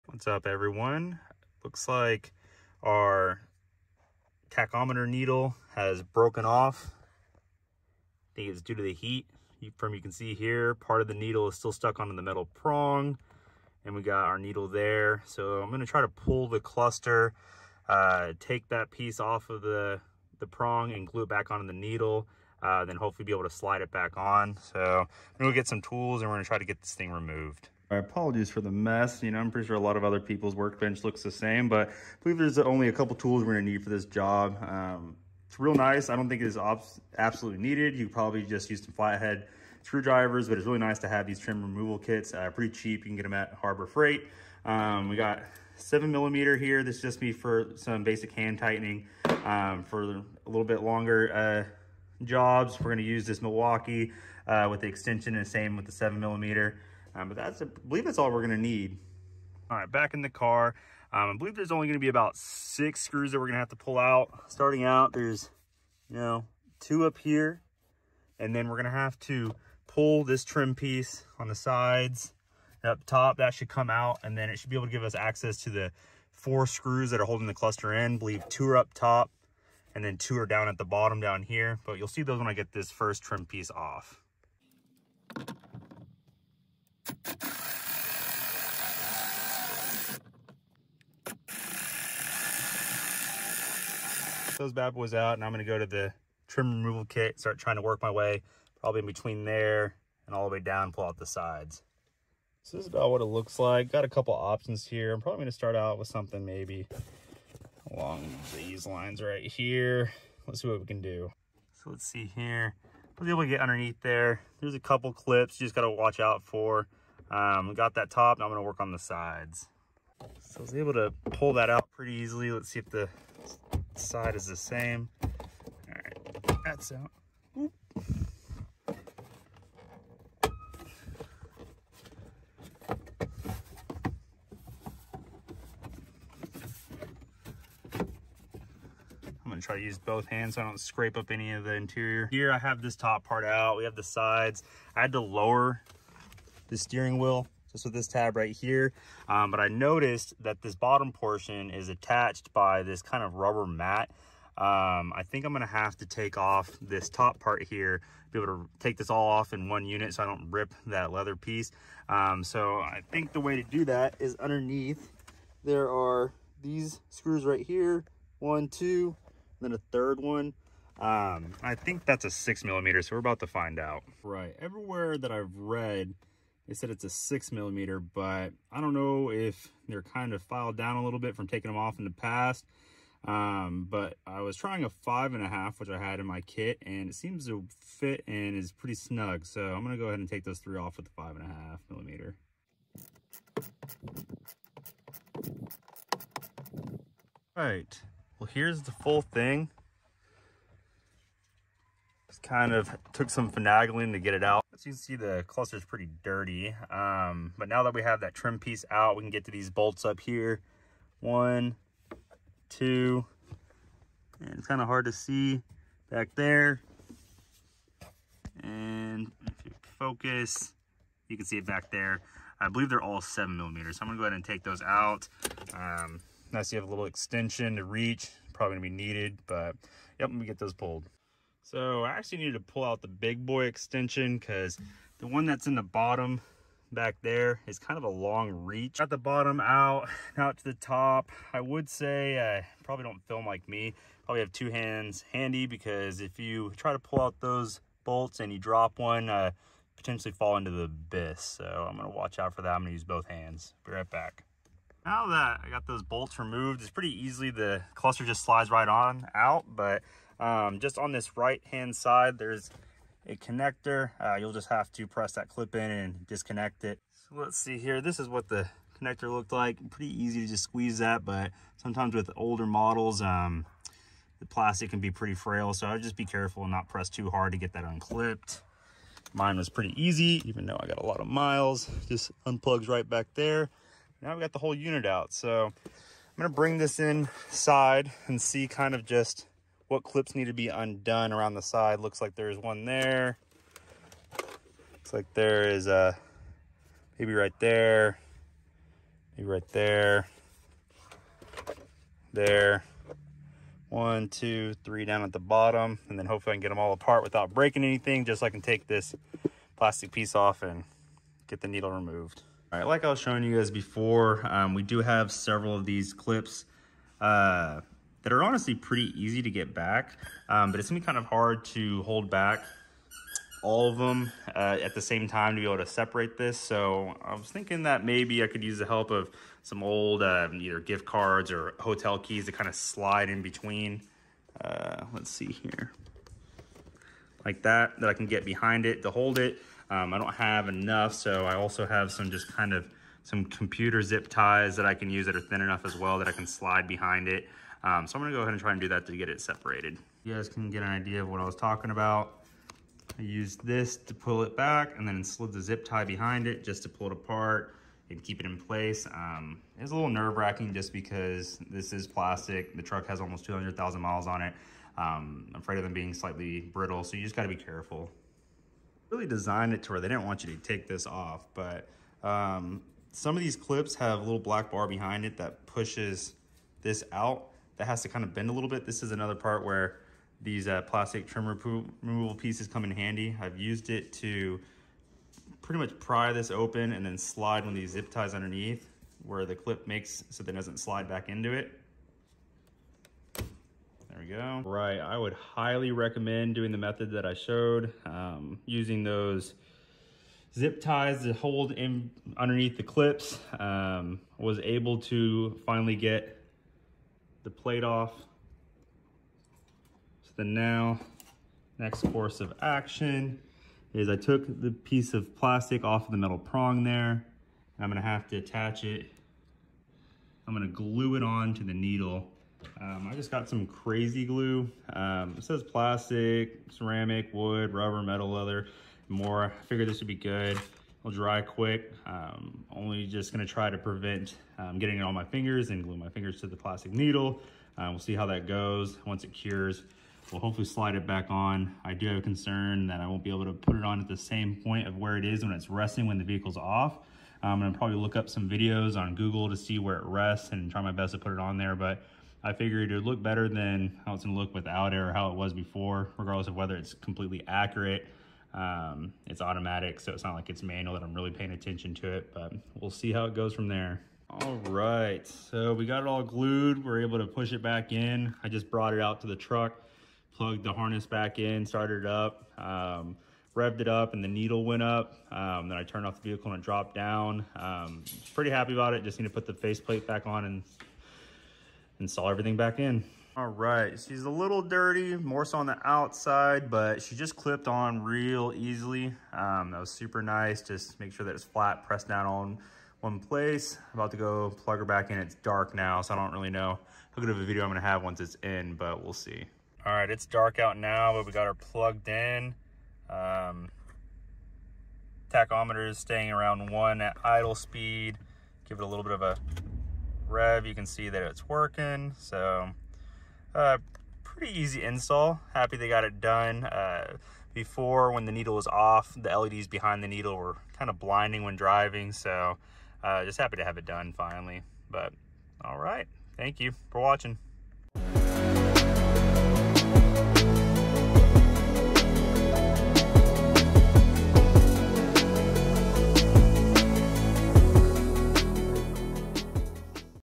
How to remove and fix F150 gauge cluster needle
In this video, I removed the gauge cluster on my 2012 Ford F150 Lariat in order to fix the broken tachometer needle. The removal of the cluster is pretty easy, but separating the clear front plastic from the rest of the cluster was the tedious part. There job overall was not very difficult and can be done with just a couple simple tools.

Extra bit not included/mentioned in the video... there is a small star bit that is needed for two screws on the back of the cluster. These need to be removed in addition to the multiple plastic tabs around the edges of the cluster.